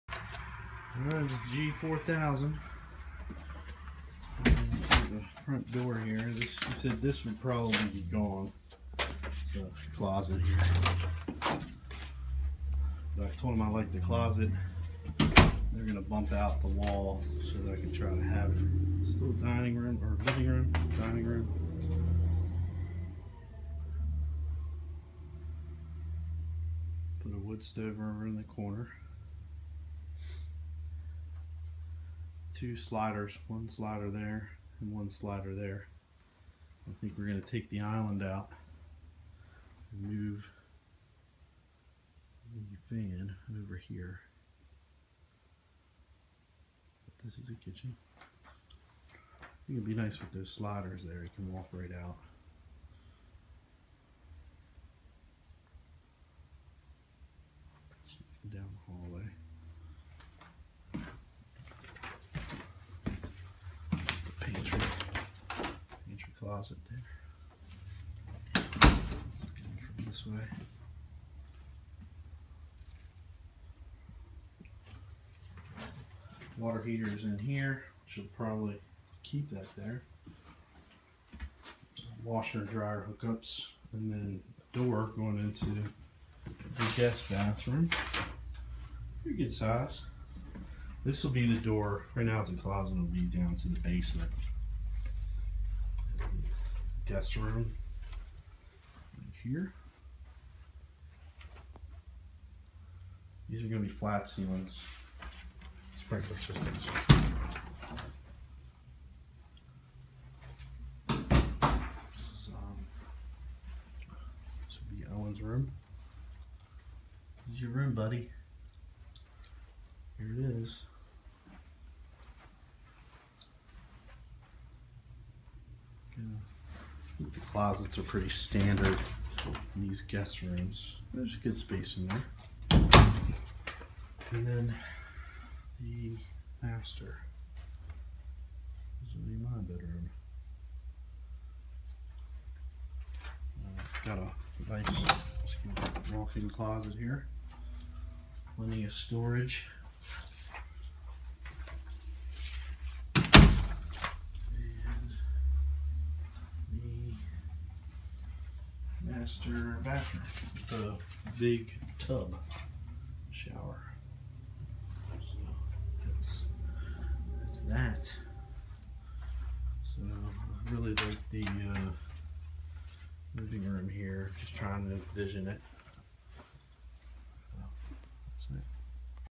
G4000 manufactured home
I went to visit the G4000 manufactured home in Thomasville Pa. This model can be upgraded to meet modular specifications.